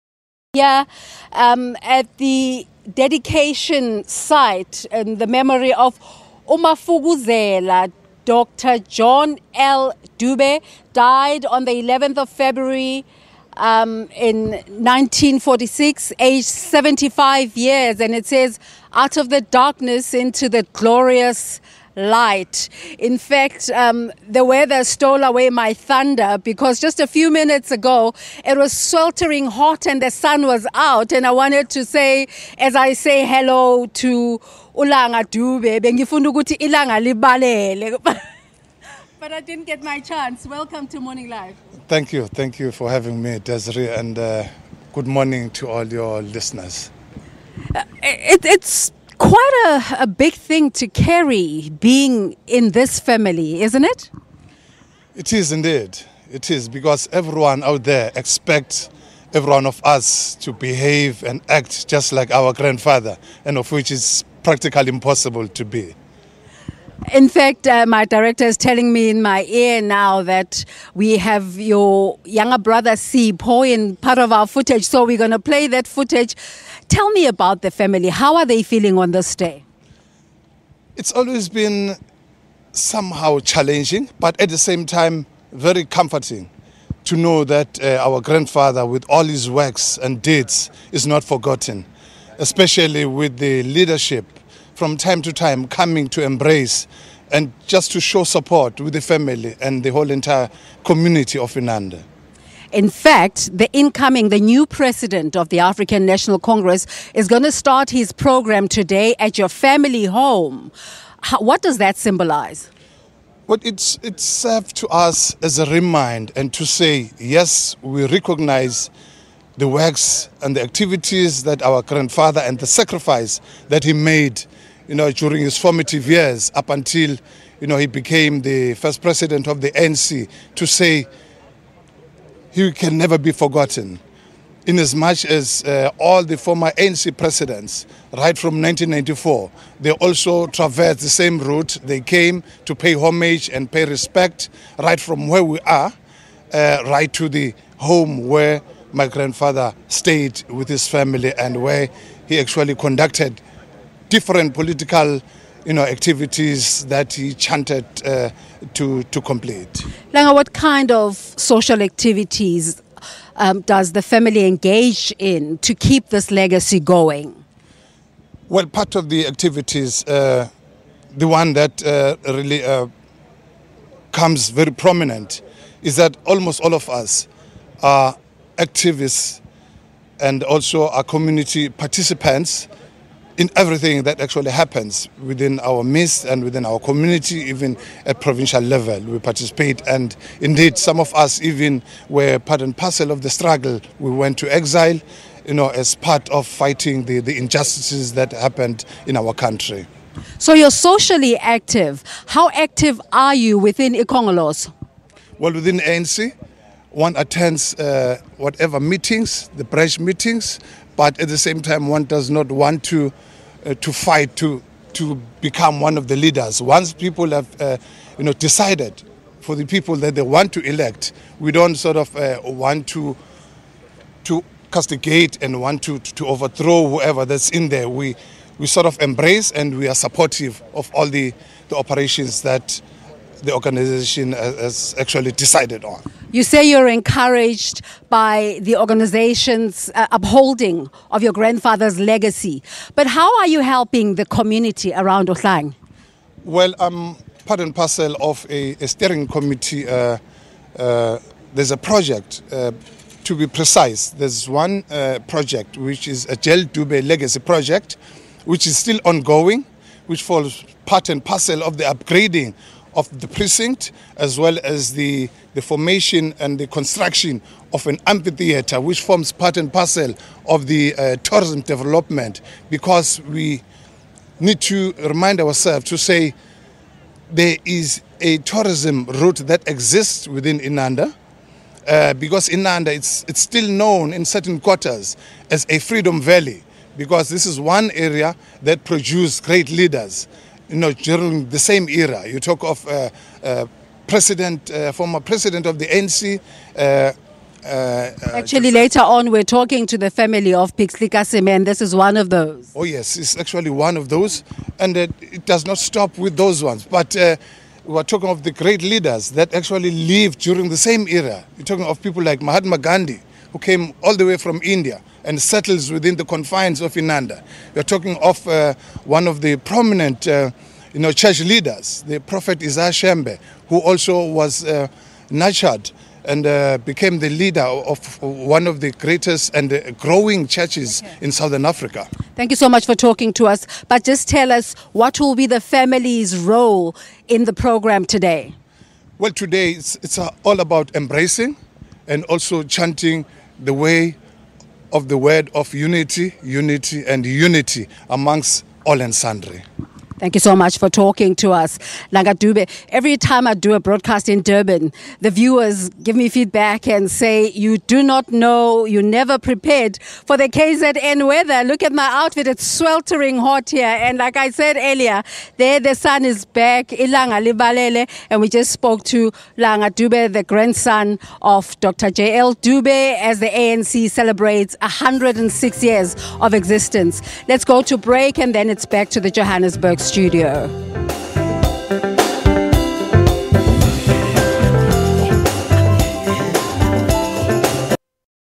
Grandson of 1st ANC president, Langa Dube on ANC 106th birthday
DR JL DUBE WAS THE FIRST PRESIDENT OF THE AFRICAN NATIONAL CONGRESS WHICH TURNS A HUNDRED AND SIX TODAY, JANUARY THE EIGHTH. HOW IS THE LEGACY OF THIS FOUNDING LEADER KEPT ALIVE. WE ARE JOINED BY THE THIRD GENERATION, HIS GRANDSON, LANGA DUBE.